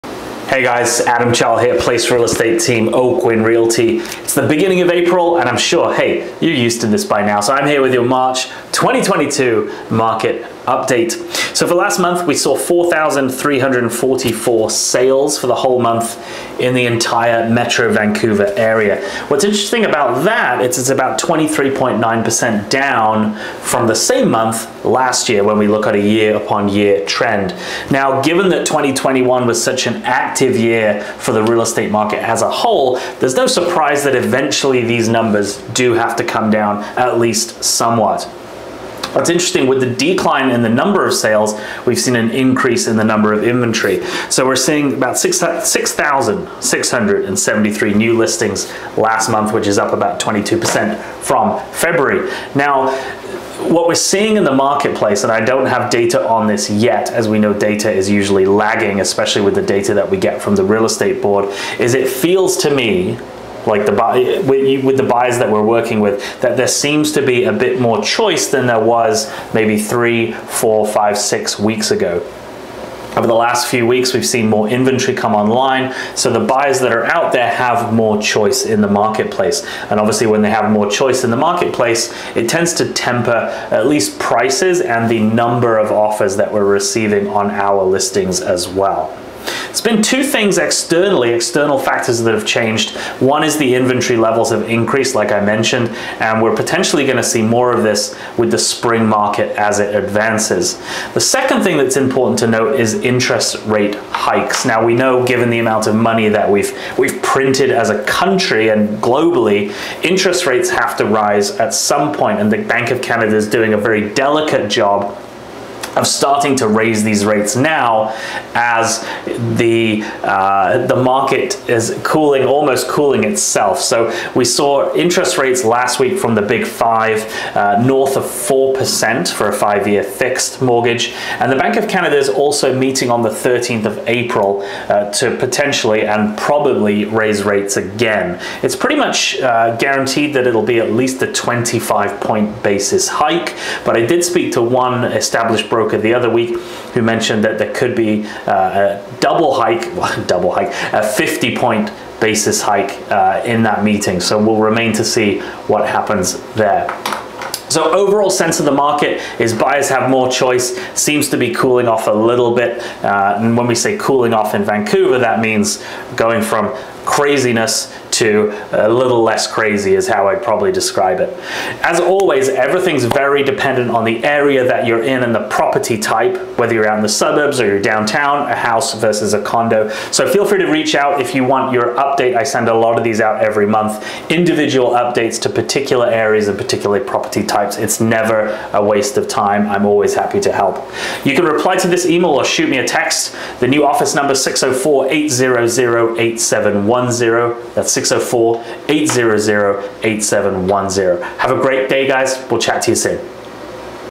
March 2022 - Vancouver Real Estate Market
While down from last year’s record numbers, home sale activity in Metro Vancouver’s housing market remained elevated in March.

The Real Estate Board of Greater Vancouver (REBGV) reports that residential home sales in the region totalled 4,344 in March 2022, a 23.9 per cent decrease from the 5,708 sales recorded in March 2021, and a 26.9 per cent increase from the 3,424 homes sold in February 2022.

Last month’s sales were 25.5 per cent above the 10-year March sales average.

Place Real Estate Team - Oakwyn Realty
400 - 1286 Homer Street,
Vancouver BC V6B 2Y5